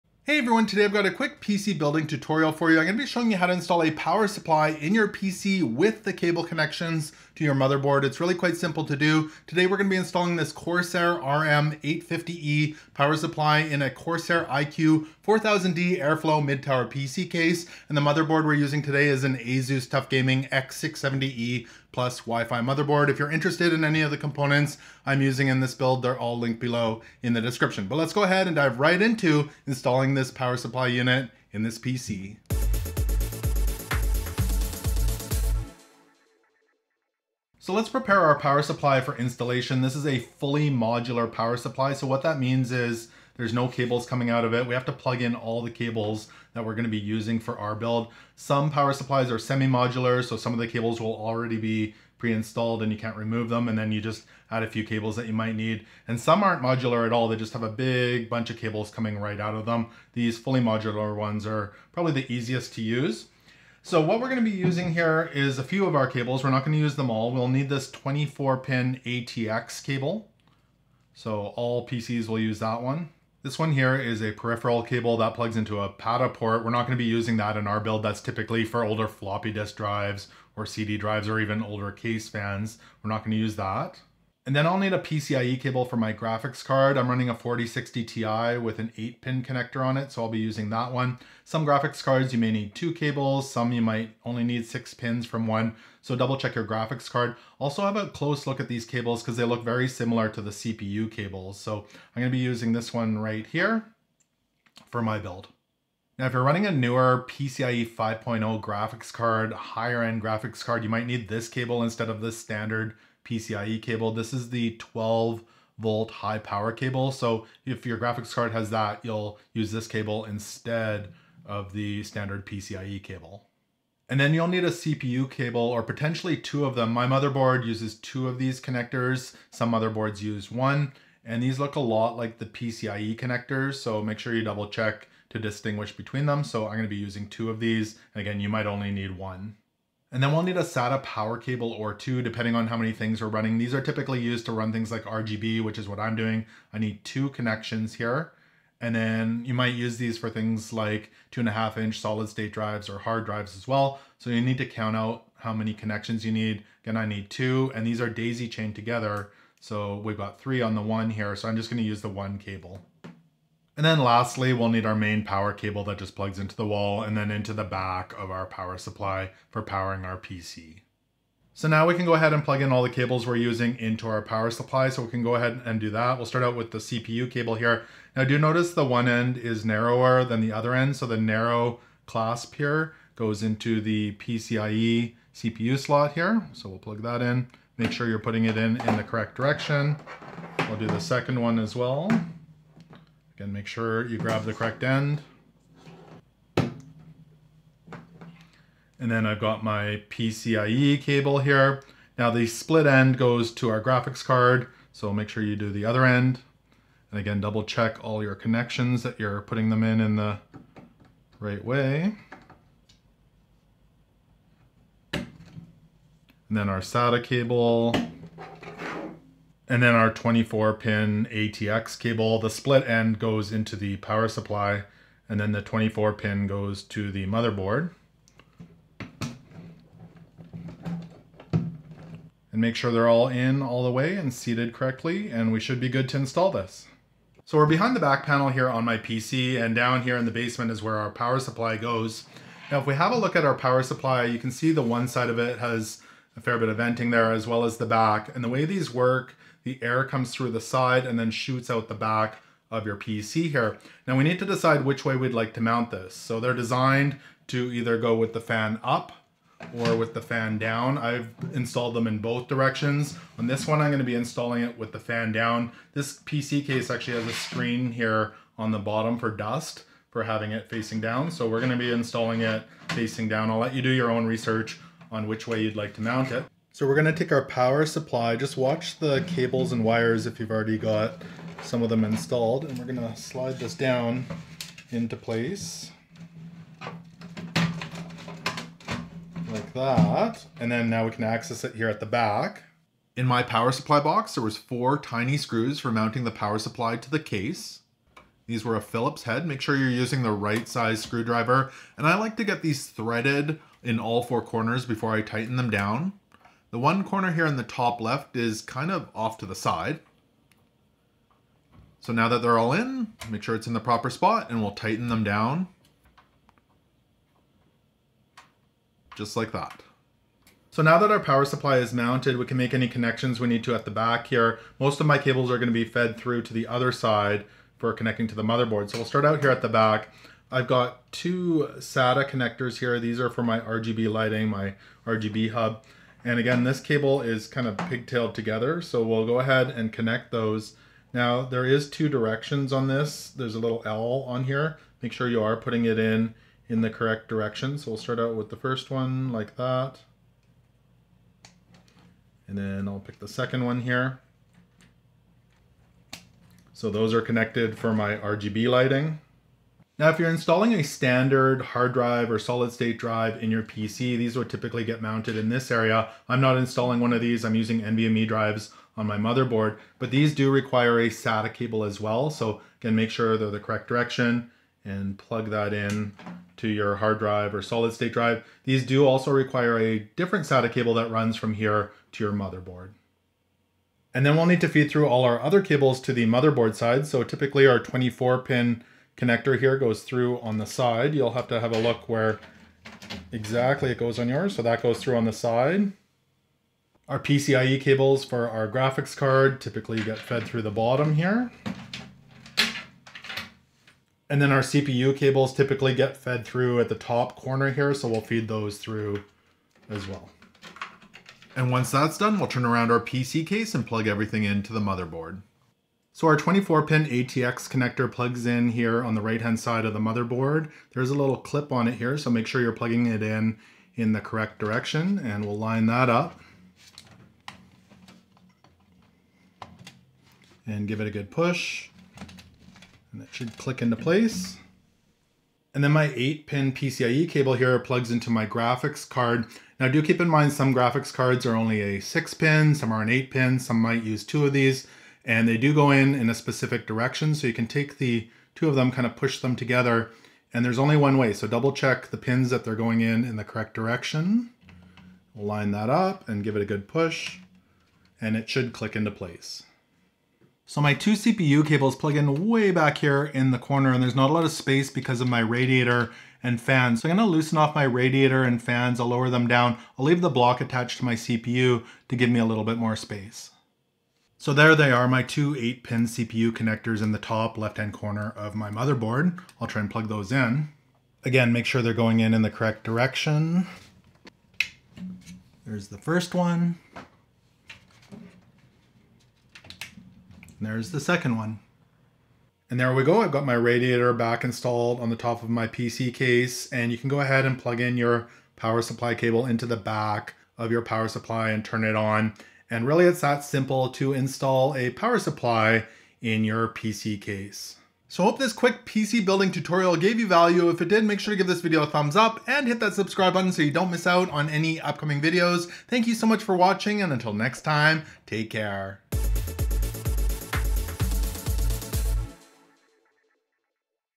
How to Install a Power Supply in a PC Step by Step (Corsair RM850e PSU)
Learn how to install a Power Supply (PSU) in your PC build with this easy, step-by-step guide! In this tutorial, I’ll walk you through selecting the right cables, securely mounting the PSU in your case, and connecting all essential power cables to your motherboard, including the 24-pin, CPU, and PCIe connectors. Whether you're using a Corsair RM850e or another model, this beginner-friendly guide will show you exactly how to install a Power Supply for a smooth and successful PC build.

Asus Tuf Gaming X670E Plus WiFi Motherboard
AMD Ryzen 9 9900X Processor
CORSAIR VENGEANCE RGB DDR5 RAM 32GB 6000MHz
WD BLACK 2TB SN850X NVMe SSD
ASUS ProArt GeForce RTX 4060 Ti Graphics Card
Corsair RM850e Power Supply
Corsair iCUE 4000D RGB Mid-Tower Case
Corsair H100x RGB Elite Liquid CPU Cooler
Corsair AF120 RGB Slim Fan
NVMe M.2 Mounting Screws Kit

⌚Time Stamps
00:00 Introduction
00:37 Selecting The Cables To Use
02:51 Preparing the Power Supply for Installation
03:56 Mounting the Power Supply in PC Case
05:20 Connecting SATA Power
06:23 Feeding the Cables through the Case
06:55 Connecting 24-pin, CPU, PCIe & Main Power Connectors
08:26 Final Thoughts